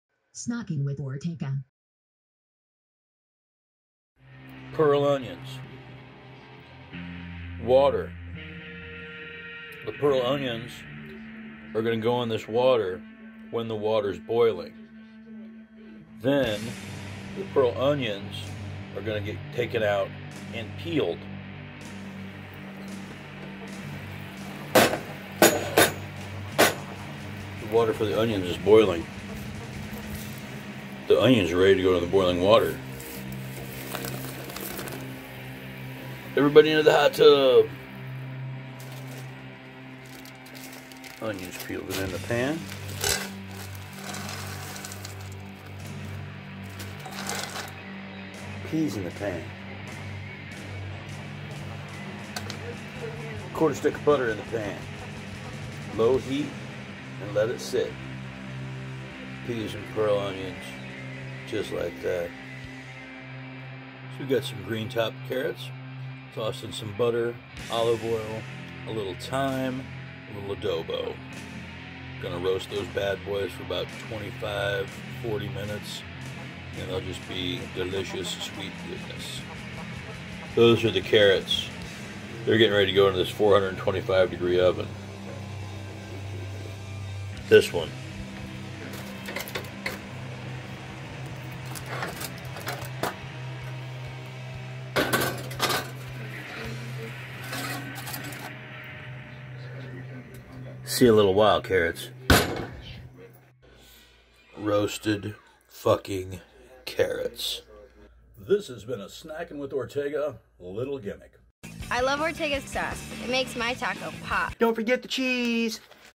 Peas & Carrots have arrived! What's pearl doing here? A Snackin' With Ortega Little Gimmick.
Welcome to Project 33! Experience Ortega LaTrobe !

The legendary pairing has come to an end. Carrots are a lone wolf and Peas has a new partner named Pearl...Pearl Onions,  and she is sweet. See how these old friends work together for the Holiday Feast.

-  Peas & Pearl Onions Ingredients
* 1 - 12 oz bag frozen peas
* 6 oz white pearl onions
* 1/4 stick butter
- Whatcha' Do: Boil water / Add onions and boil for 2-3 minutes / drain / Peel Onions / Add to large pan / Add peas / Add Butter / Cook over low heat until butter is melted and peas are fully heated.

For the green top carrots: Preheat oven to 425 / Trim tops / Toss in butter and olive oil / Season with Thyme and Adobo or garlic powder, onion powder, pepper / Place in baking dish-pan / Roast for 30-45 minutes

*Check out Ortega's Hotplate: A Talking Show , also available audio only on Spotify, Google and other podcast platforms
*Shout Out to patrons Scott Jones & bWb Dennis
*Shout Out to Johndope for podcast support